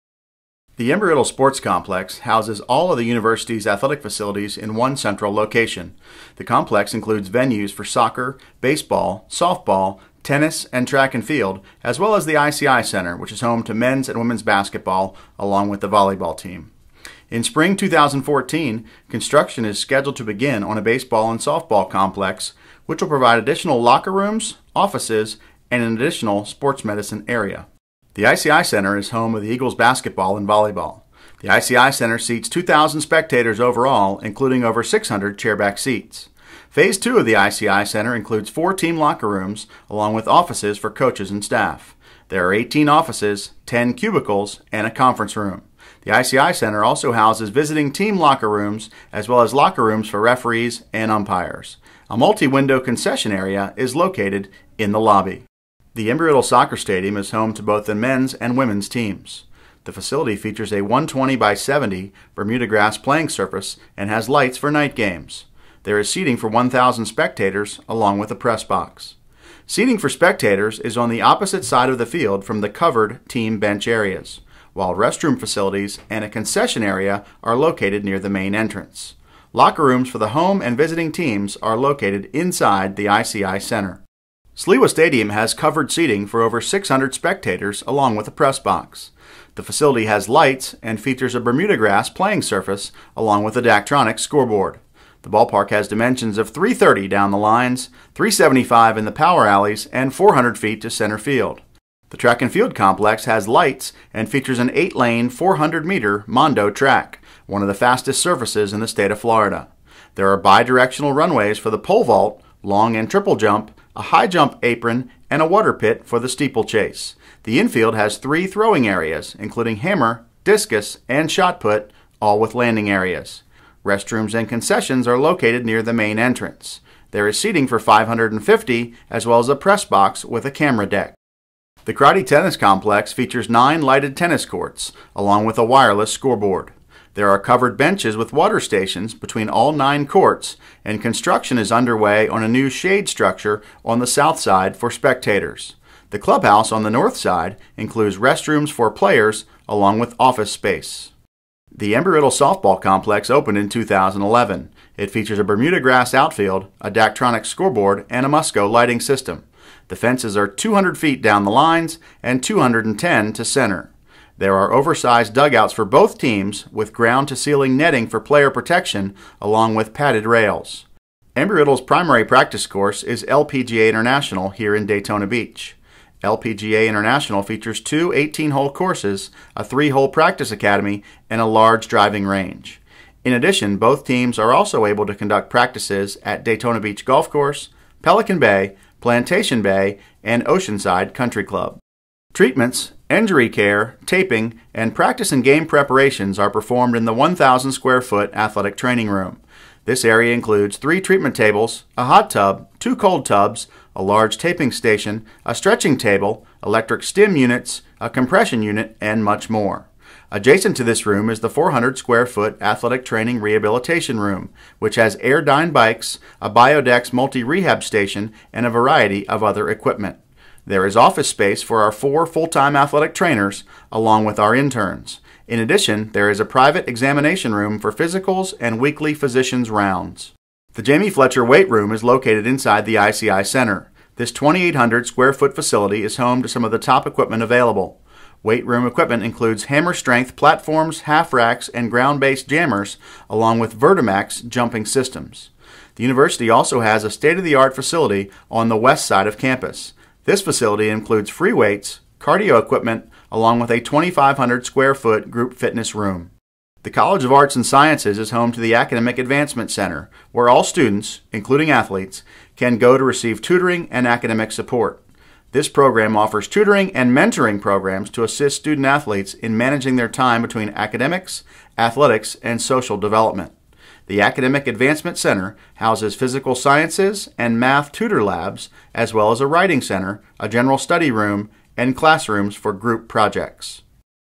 ERAU Facilities video
Here's a brief look at the athletic facilities of Embry-Riddle from 2014.